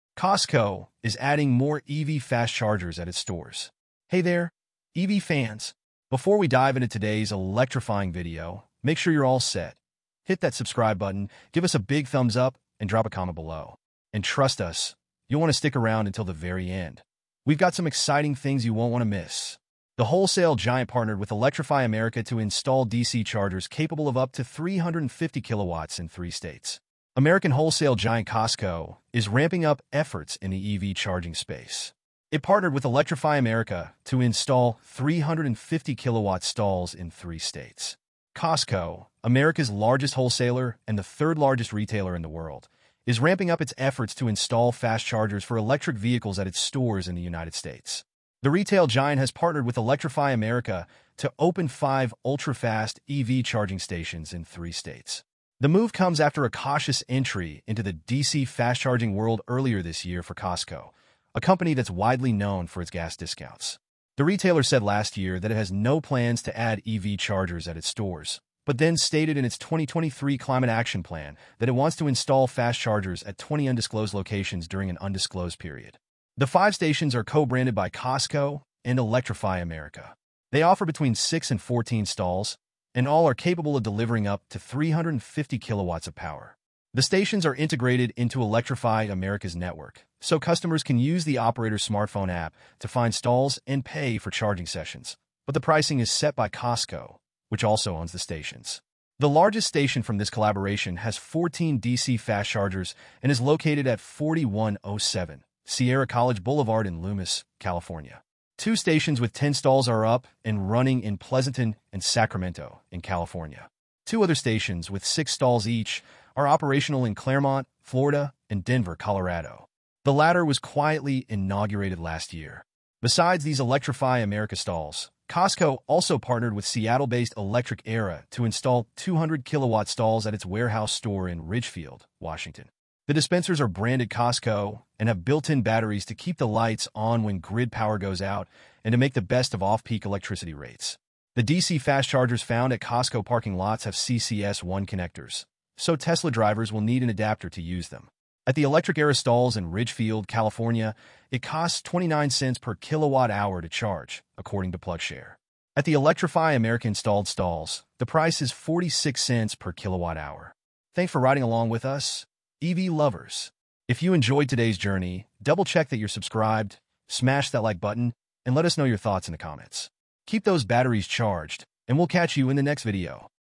Costco Is Adding More EV Fast Chargers At Its Stores
⚡ Costco Expands EV Fast Charging Network with Electrify America!

Costco is stepping up its EV charging game by partnering with Electrify America to install ultra-fast DC fast chargers capable of up to 350 kW in three states. This expansion includes five major locations, offering 6-14 stalls per station, with the biggest site in Loomis, California featuring 14 chargers.

🔋 Key Details:
✅ States Covered: California, Florida, Colorado
✅ Charging Speeds: Up to 350 kW (CCS1)
✅ Pricing: Costco sets rates at competitive levels
✅ Bonus: Electric Era stations in Washington with backup batteries

📌 Watch now to learn how Costco is reshaping EV charging convenience at its U.S. stores!